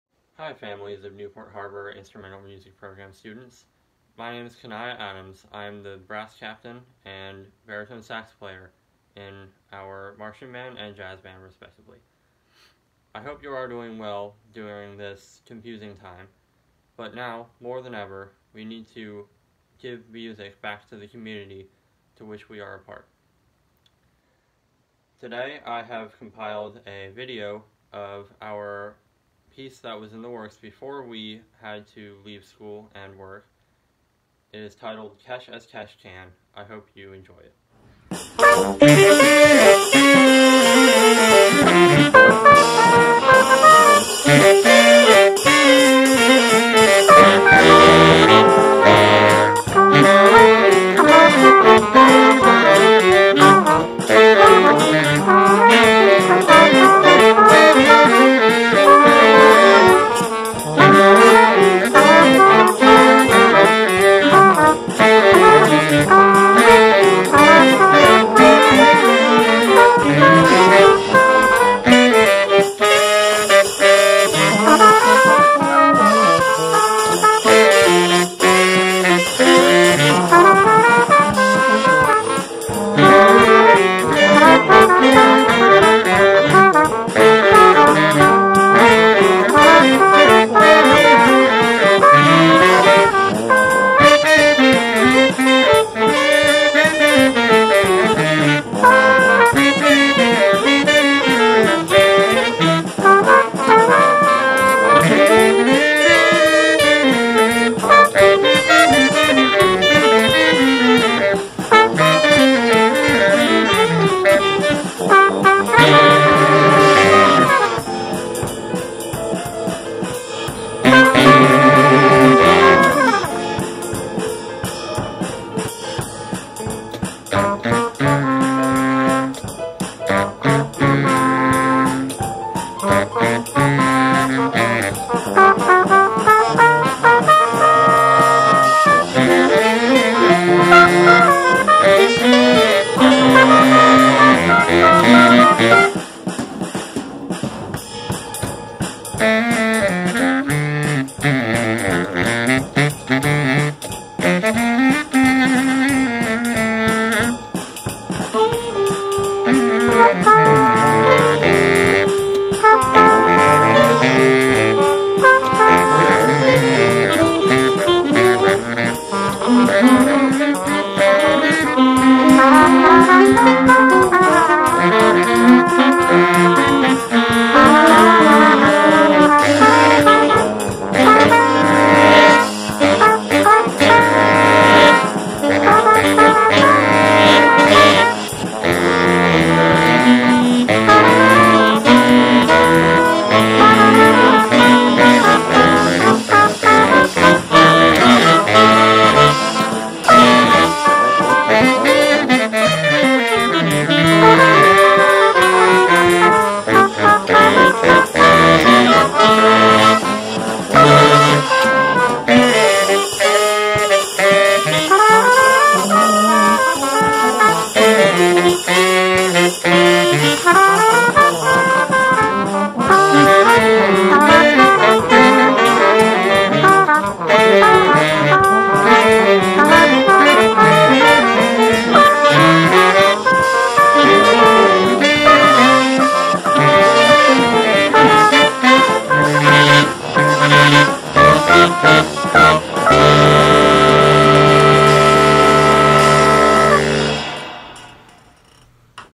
Newport Harbor High School Jazz Ensemble Virtual Ensemble
Virtual Ensemble performance of Spring Concert music.